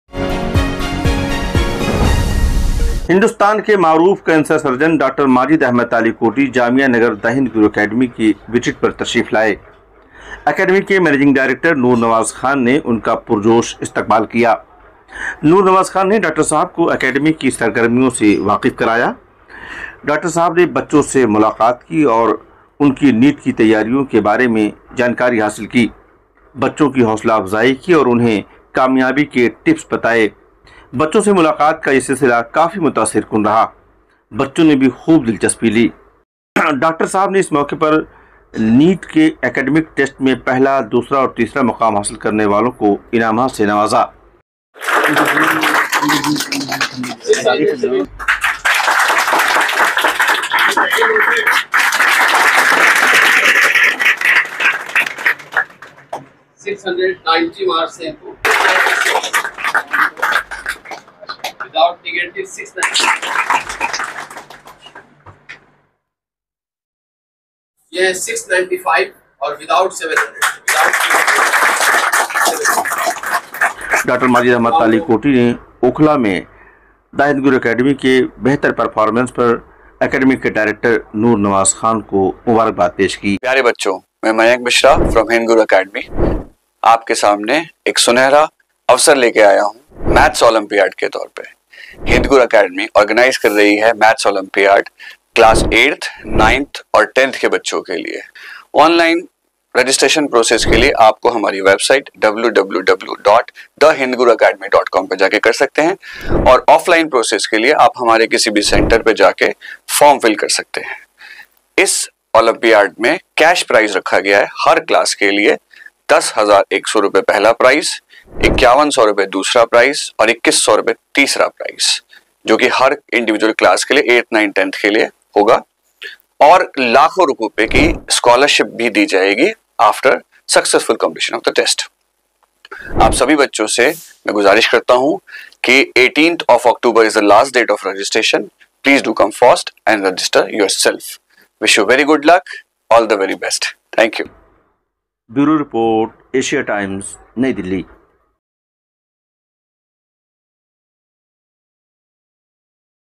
देश के जाने माने कैंसर सर्जन Dr. Majid Talikoti ने The Hind Guru Academy विजिट किया ll NEET - 2025
NEET - 2025